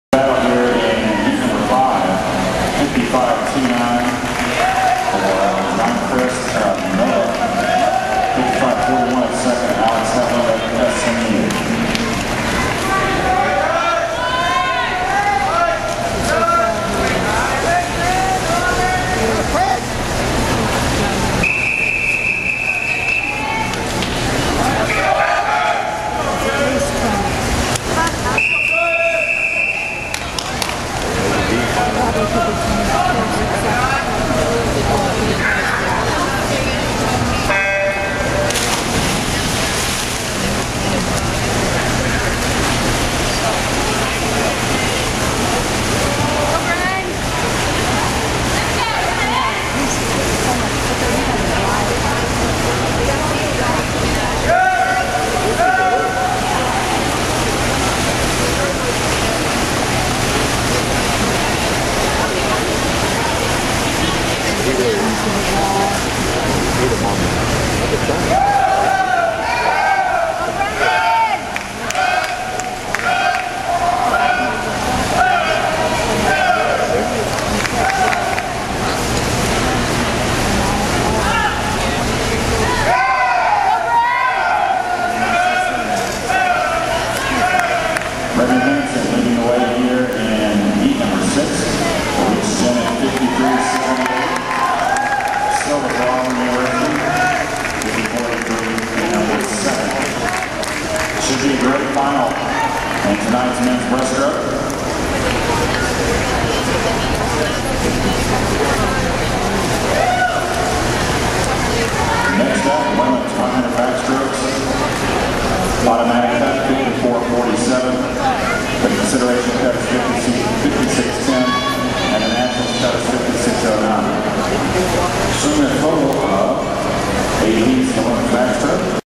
Brendan Hansen 100 Breast Prelim at American Short Course 07
Brendan Hansen swims the 100 Breaststroke Prelim at American Short Course Championships in Austin, Texas - February 23rd, 2007